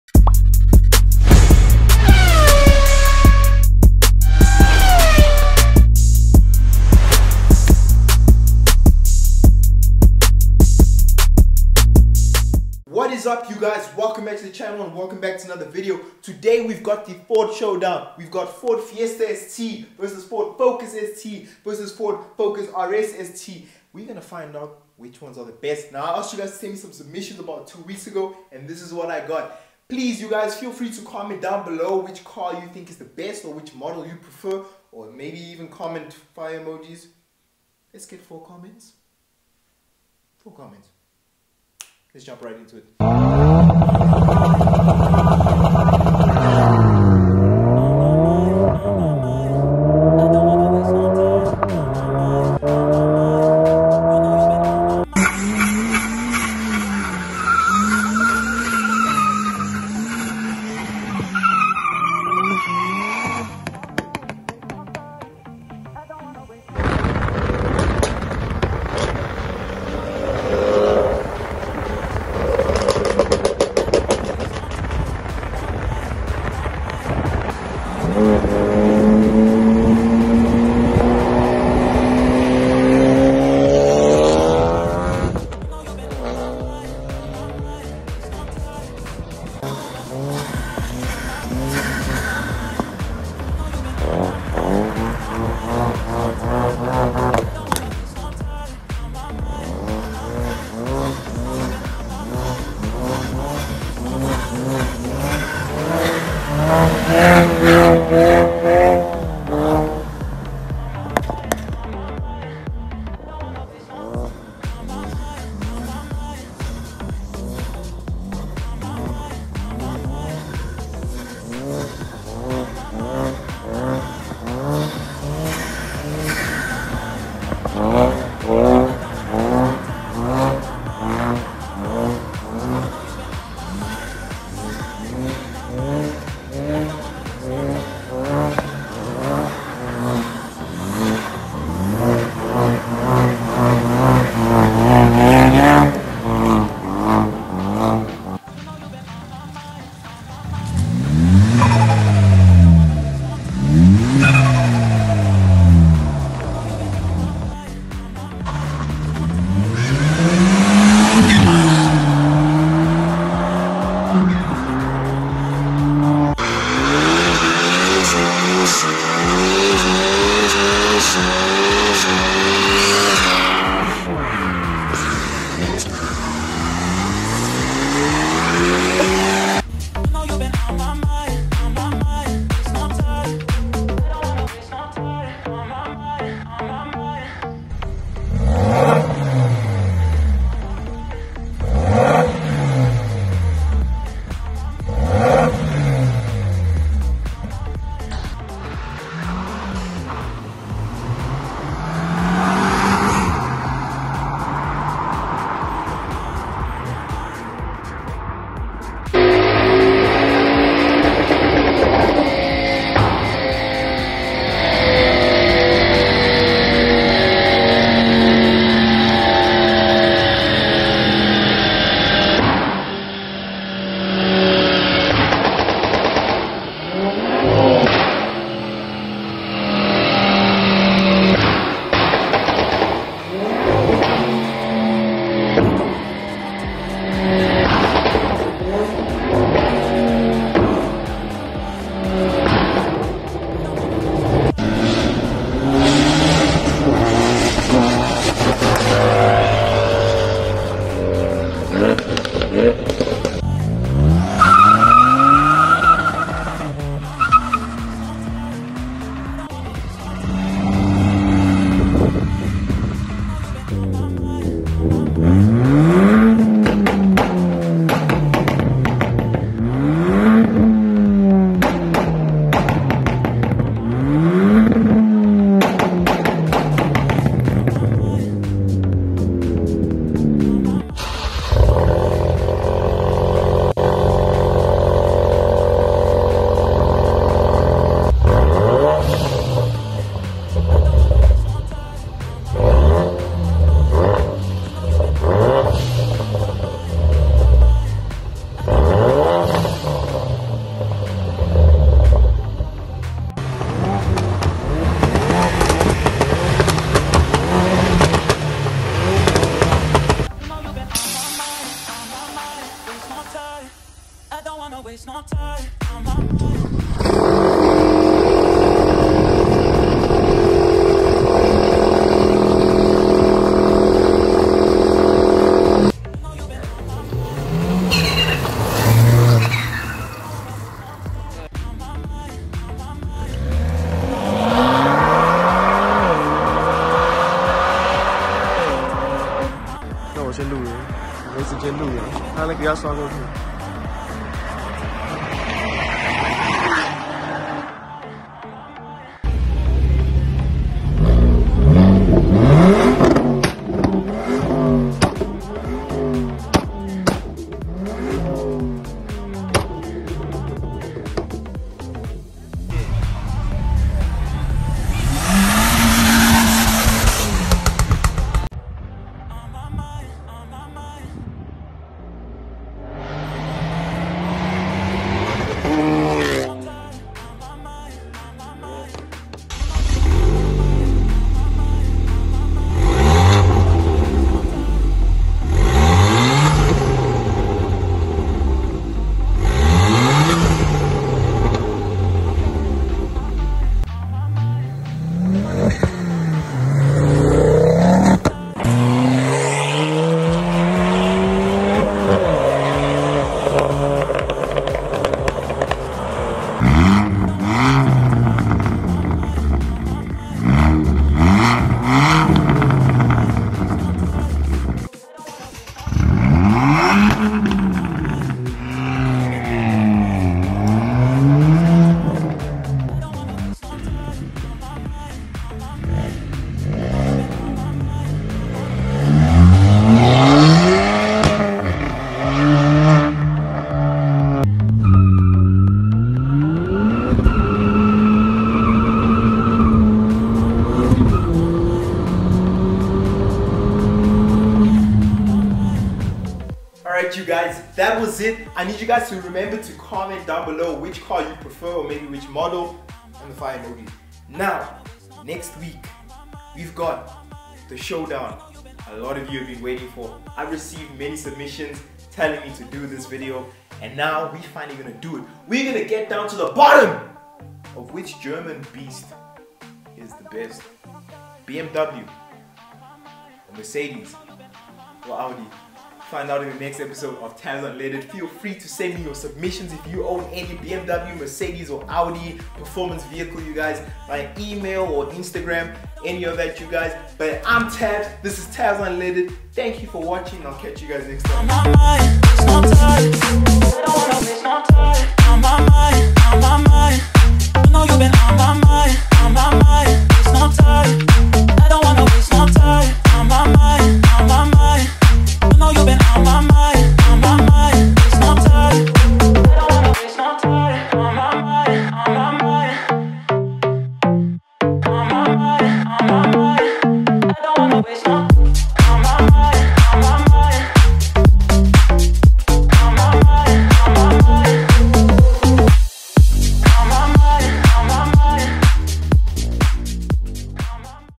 Ford Focus RS vs Ford Focus ST vs Ford Fiesta ST
Lets settle the debate, which Ford performance hatchback is the best?
This video showcases both modified and stock cars. which would you choose?